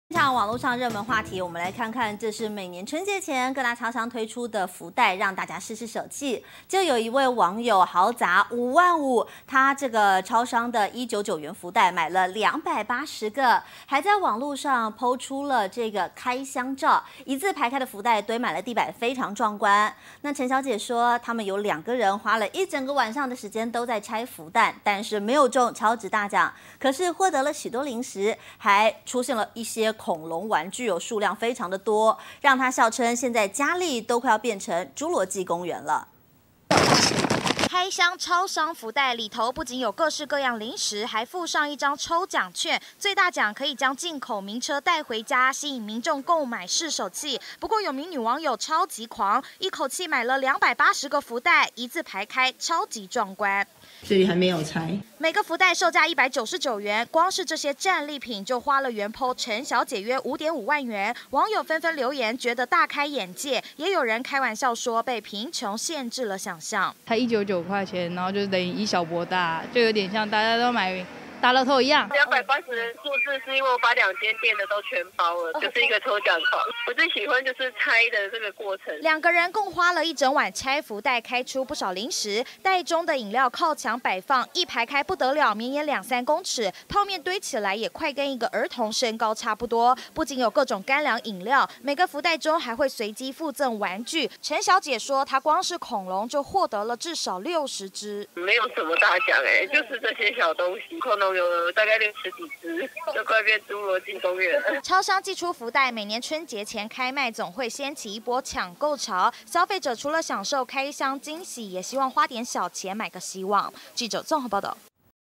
買280個福袋PO成果照 網:被貧窮限制想像
每年春節前，各大超商都會推出福袋，讓民眾試試手氣。有名網友陳小姐，就砸了約5.5萬元，買了280個超商福袋，並分享開箱照，一字排開的福袋堆滿地板，超級壯觀，陳小姐說，兩個人共花了一個晚上拆福袋，雖然沒有中超值大獎，但獲得了許多零食，甚至還開出不少恐龍玩具，讓她笑稱，家裡快變侏儸紀公園了。